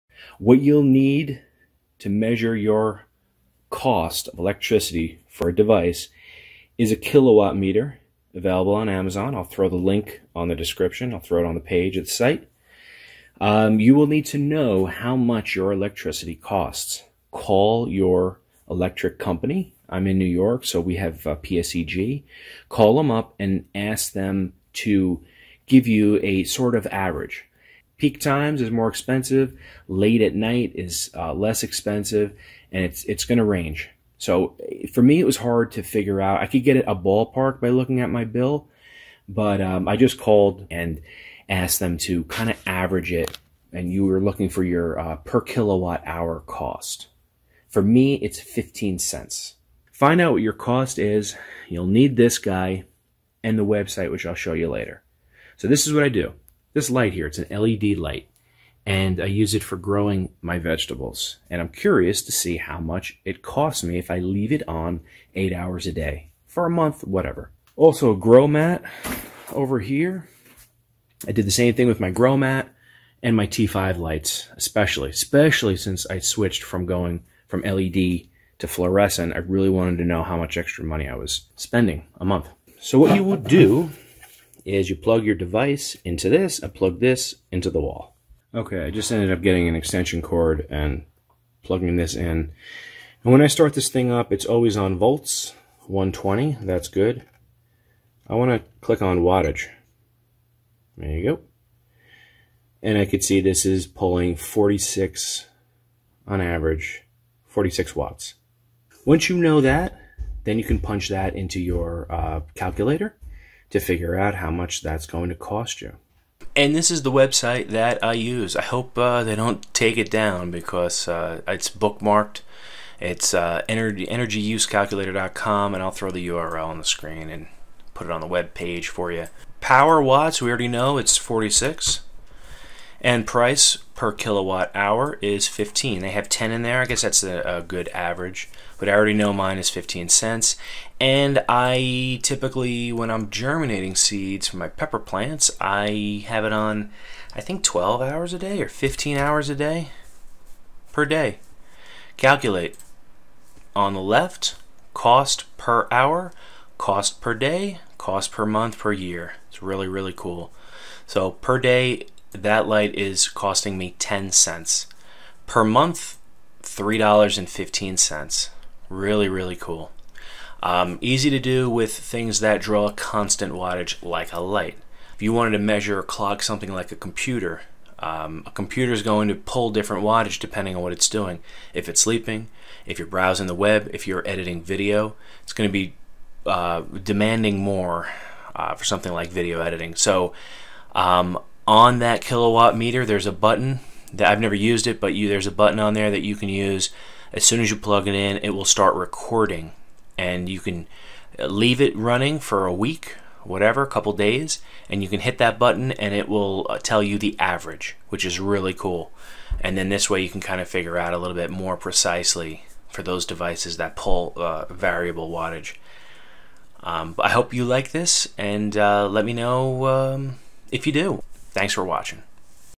How to easily measure power consumption (& cost) of an electronic device
Step 1: You'll need to get a Kill A Watt meter
Step 2:  Look at your bill or (better) call your local power company and ask them for your average per kilowatt hour power usage.  It should be somewhere around 12 cents.
Step 3: Go to this website and punch in the numbers!

If you've ever wondered how much an electronic device costs to run, this is the easiest way I have found.  You can measure anything that has an outlet: computers, lights, microwaves, and especially those devices in standby mode (vampire power) like home theater receives and tvs.